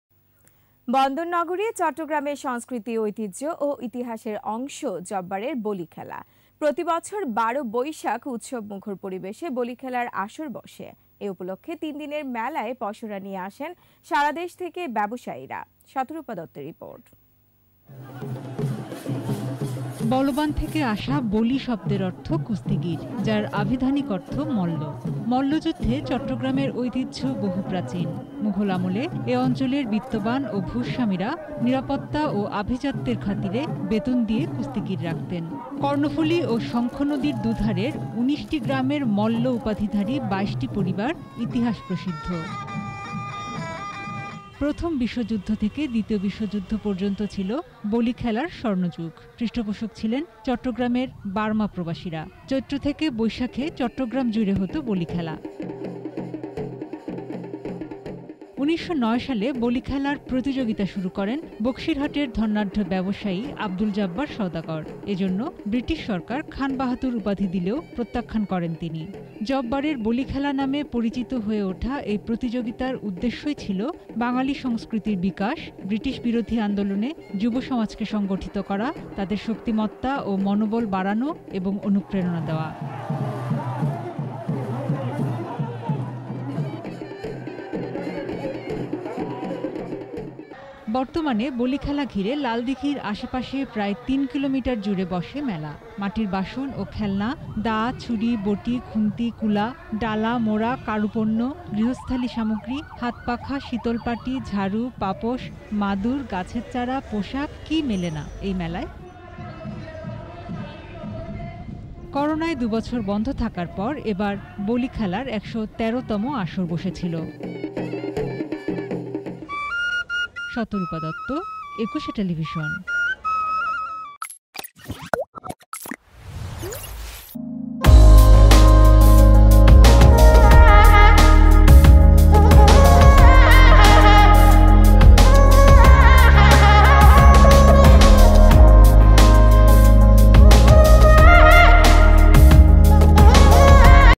২ বছর পর চট্টগ্রামে আবারো জব্বারের বলীখেলা | Jobbarer Boli Khela | ETV News
বন্দরনগরী চট্টগ্রামের সংস্কৃতি, ঐতিহ্য ও ইতিহাসের অংশ জব্বারের বলীখেলা। প্রতি বছর ১২ বৈশাখ উৎসবমুখর পরিবেশে বলীখেলার আসর বসে। এ উপলক্ষে তিনদিনের মেলায় পসরা নিয়ে আসেন সারাদেশ থেকে ব্যবসায়ীরা। শতরূপা দত্তের রিপোর্ট।